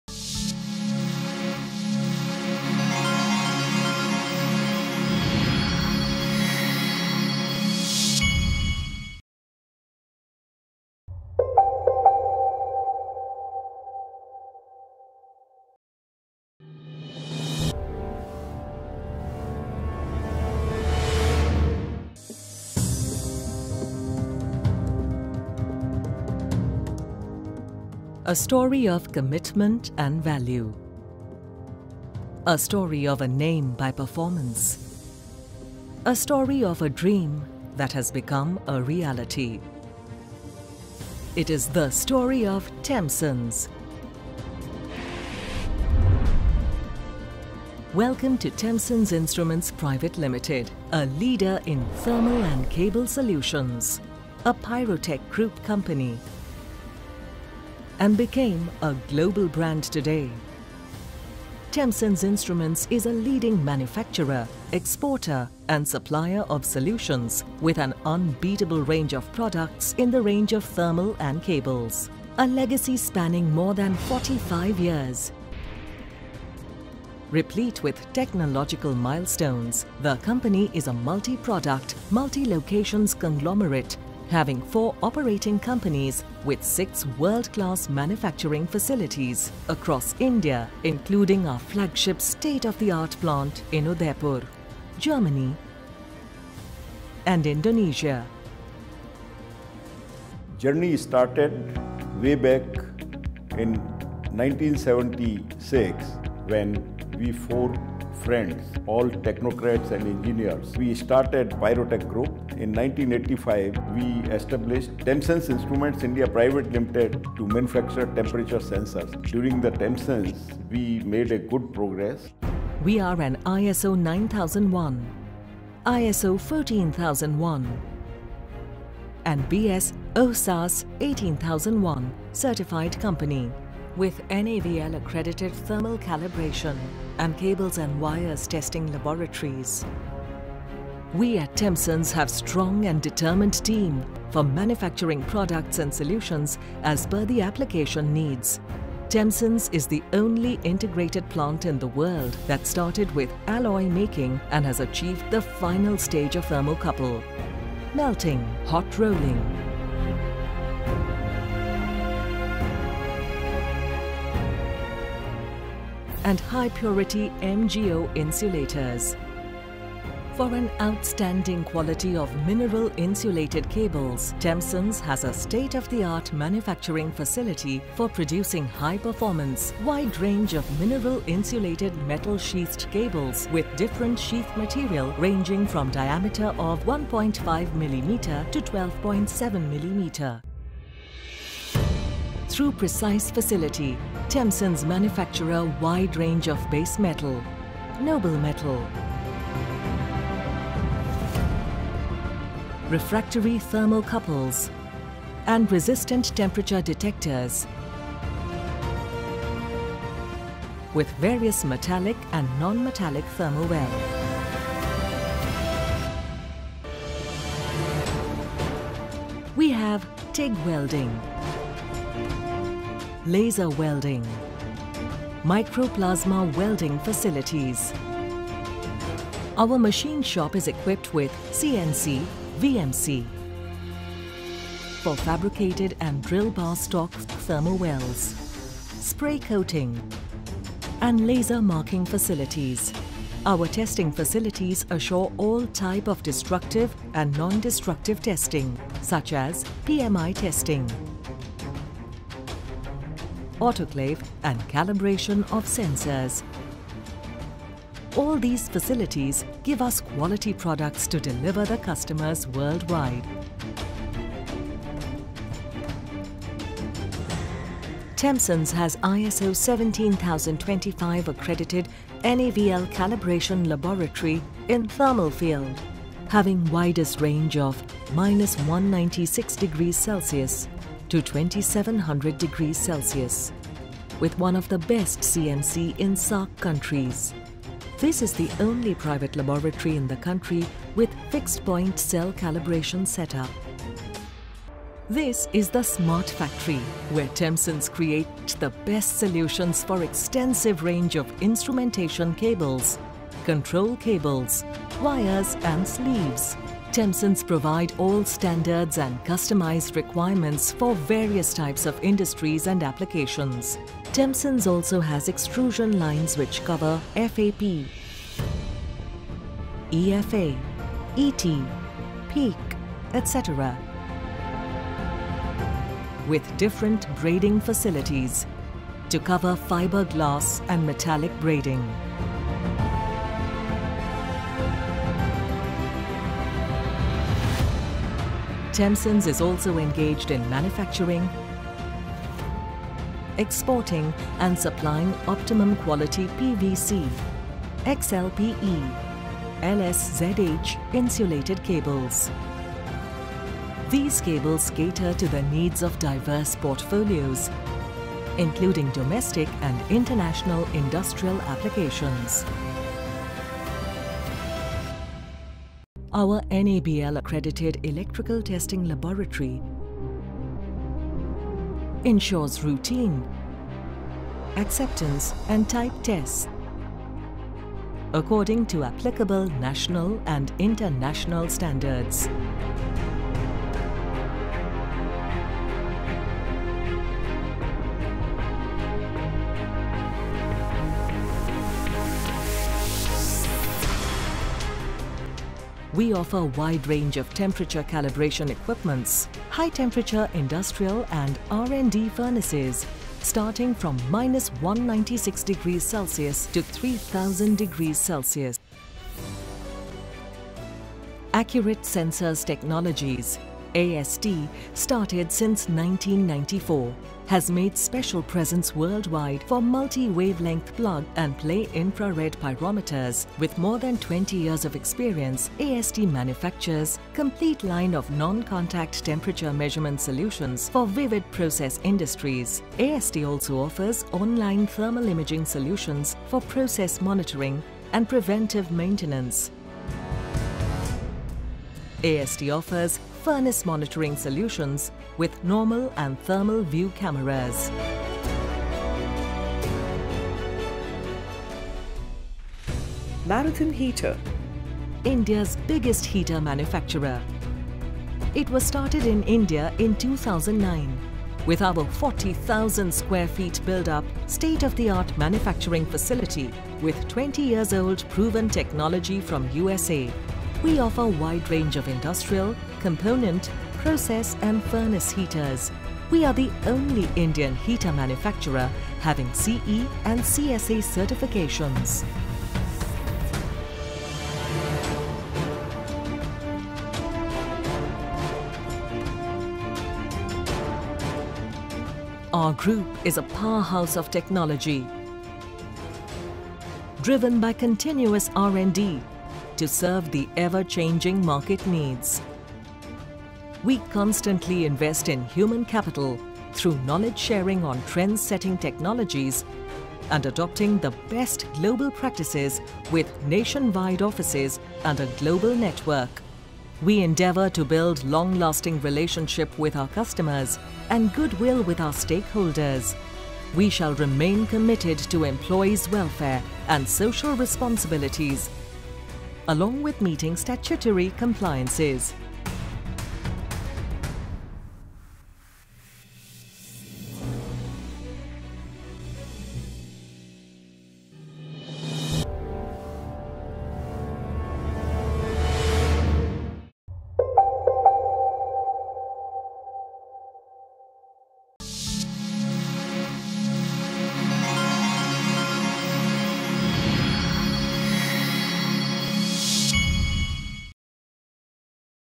Tempsens Instruments - A leader in thermal & cable solutions-Sensors,Cables Manufacturers& Suppliers
Engineered Thermal Products & Solutions

A Brief of Tempsens Instruments - An Integrated Thermal & Cable Plant, Production Facilities, Calibration & Testing Facilities.
Tempsens Instruments is a leading manufacturer and supplier of Thermal & Cable Solutions with manufacturing units in India, Germany and Indonesia. Tempsens Corporate Video shows a brief about our corporate culture.
 An Integrated Thermal & Cable Plant, Production Facilities, Calibration & Testing Facilities.
Tempsens Instruments is a leading manufacturer and supplier of Thermal & Cable Solutions with manufacturing units in India, Germany and Indonesia.